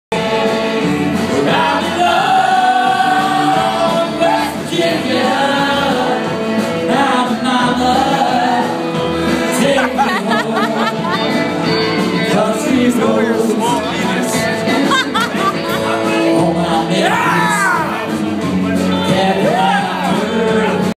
New Years Bull Riding
Tyler goes first and ends with over 28 seconds